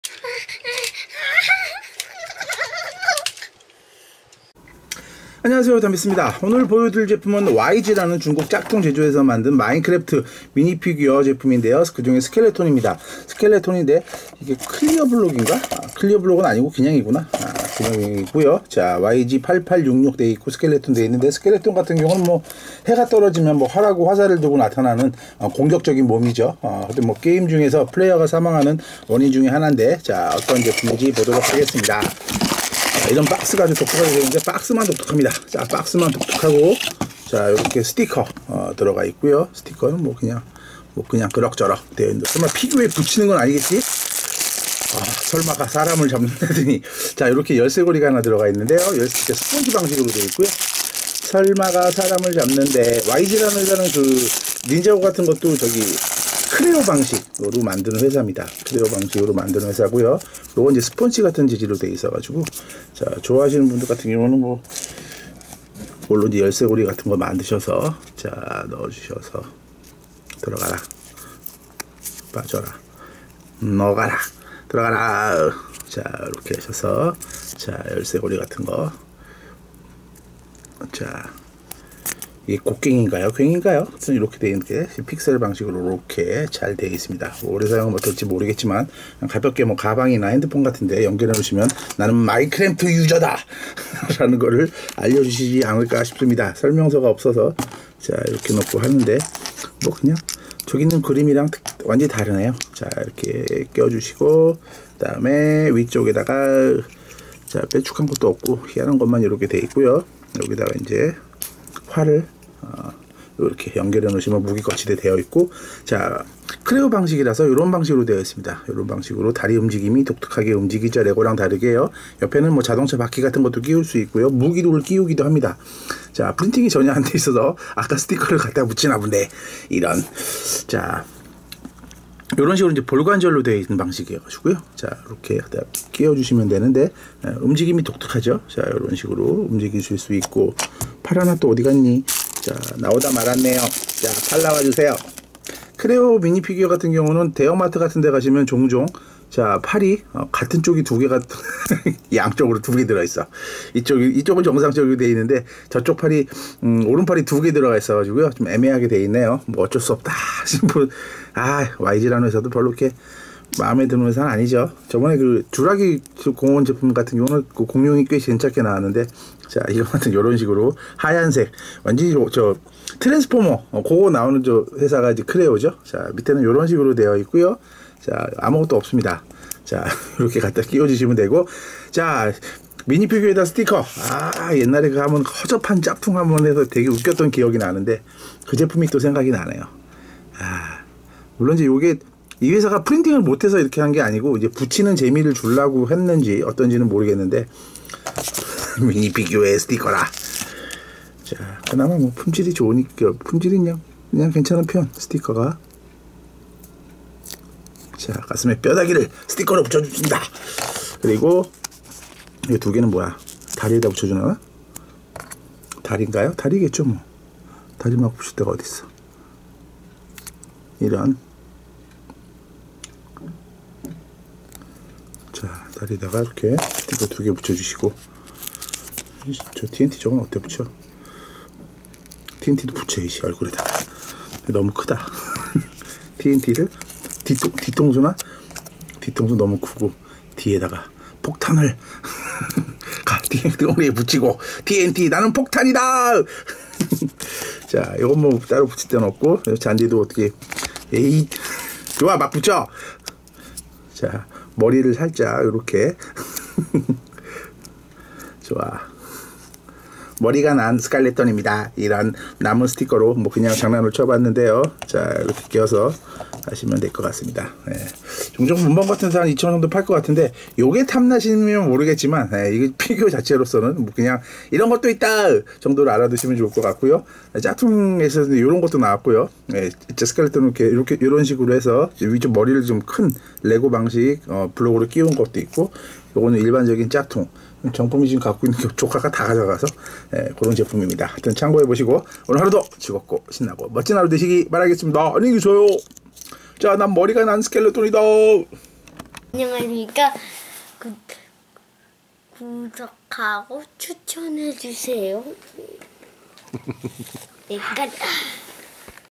yg 마인크래프트 스켈레톤 미니피규어 중국 레고 짝퉁 리뷰 Lego knockoff Minecraft skeleton Clear block minifigure
레고 방식보다는 크레오 방식의 미니피규어를 만드는 중국 짝퉁 제조회사인 YG에서 이번에는 독특한 박스에 레고 마인크래프트 미니피겨와 열쇠고리를 판매를 하는데, 미니피겨는 품질은 좀 안좋고, 오히려 키체인이 더 마음에 드네요.

스티브, 다이아몬드 스티브, 펌킨, 자이언트, 엔더맨, 블레이즈 등이 들어있습니다.

그중에 해가 떨어지면 활과 화살을 들고 나타나는 스켈레톤은 공격적 몹 플레이어를 항상 선제 공격하는 몹인데, 게임 내에서 플레이어가 사망하는 원인중에 하나입니다.